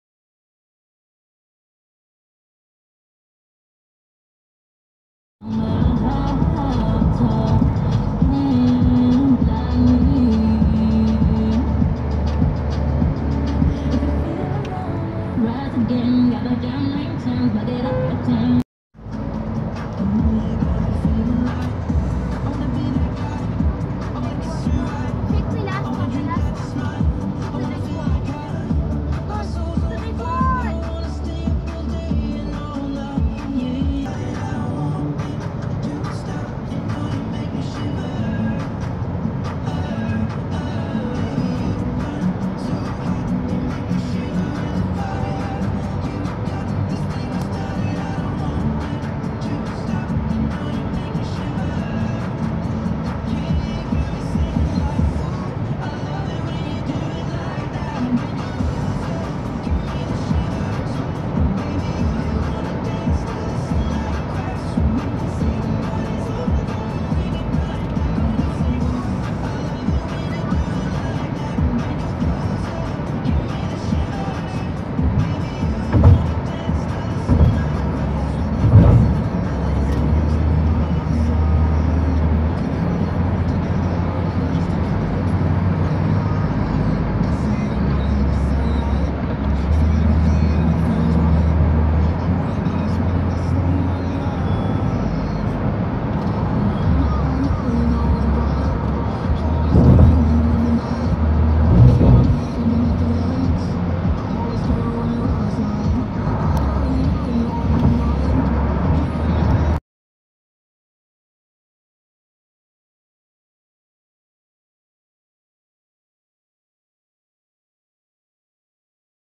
buhay ofw papuntang hospital para sa 3rd vaccine booster
buhay ofw puntang hospital para sa vaccine for booster